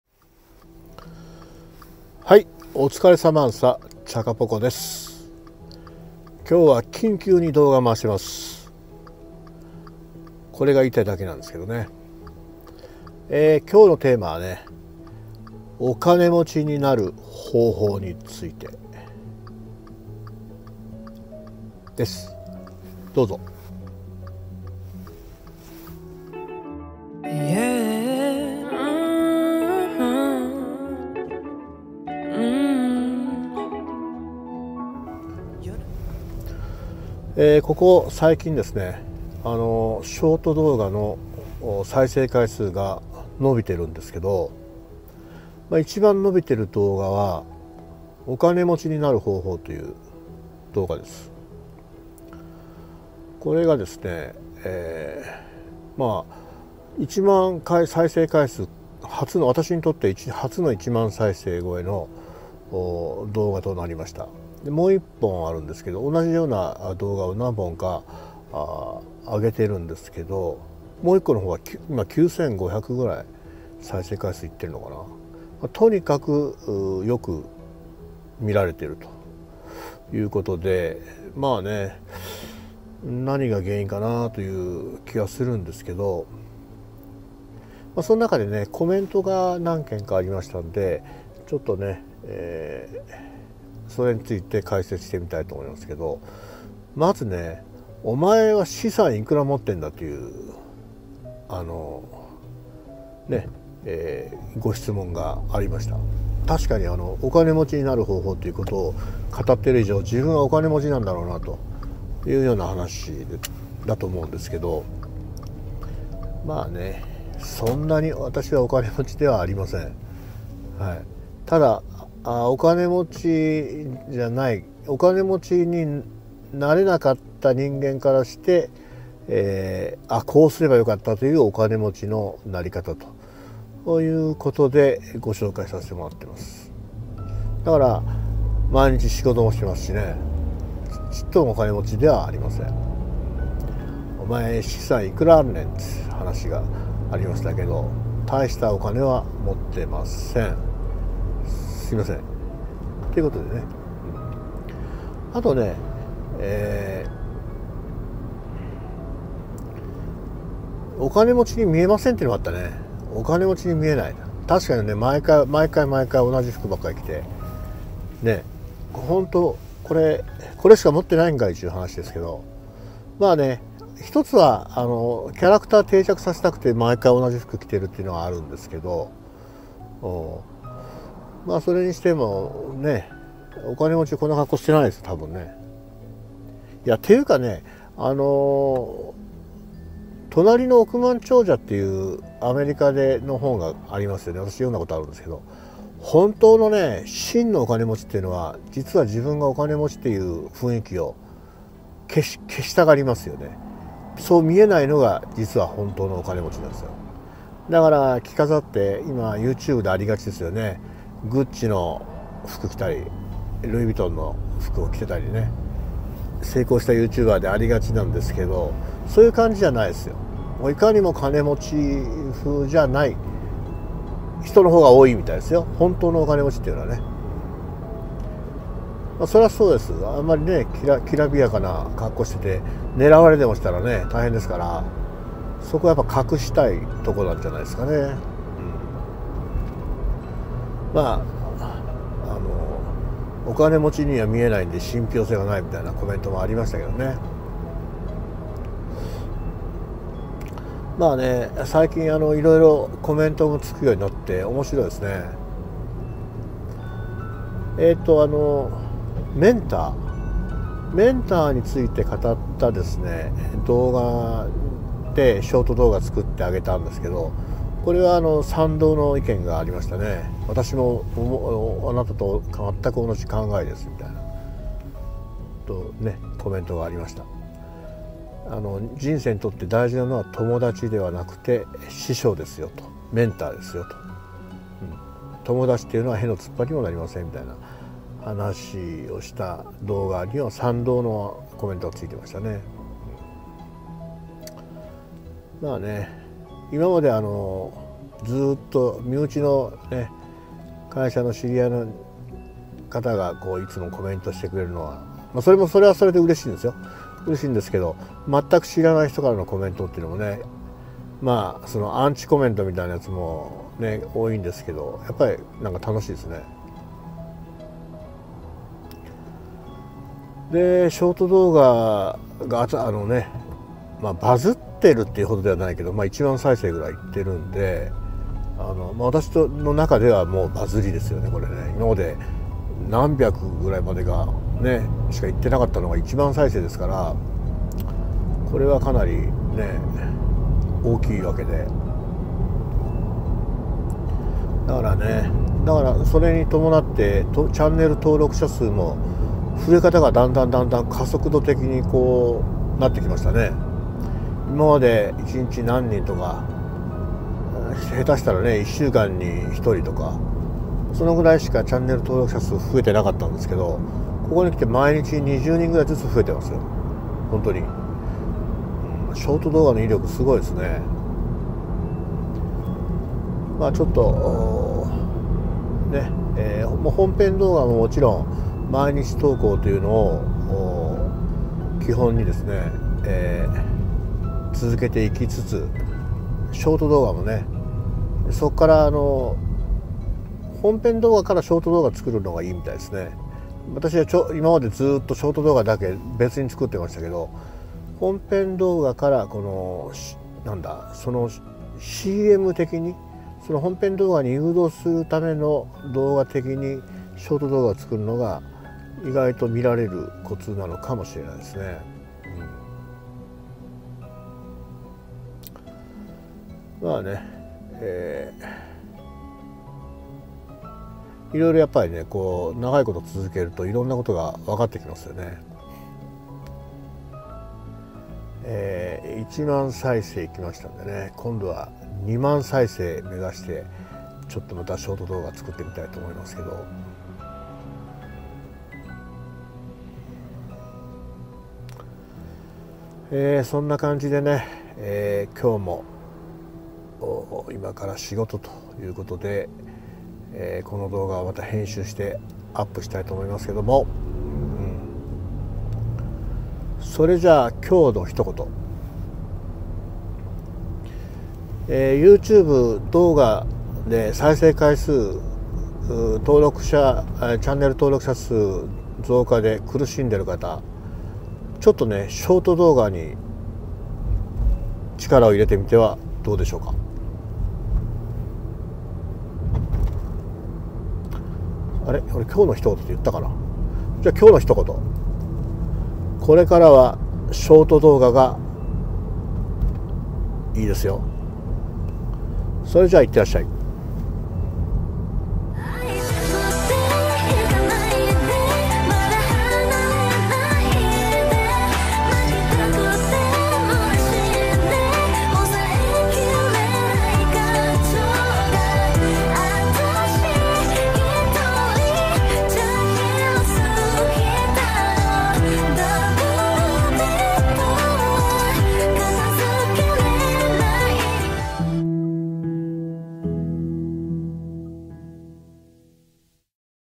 【お金持ちになる方法】偉そうにお金持ちになる方法(ショート動画)を語っているが、本当にお金持ちなのか語ってみた。【字幕入り】
▼ダブルベッド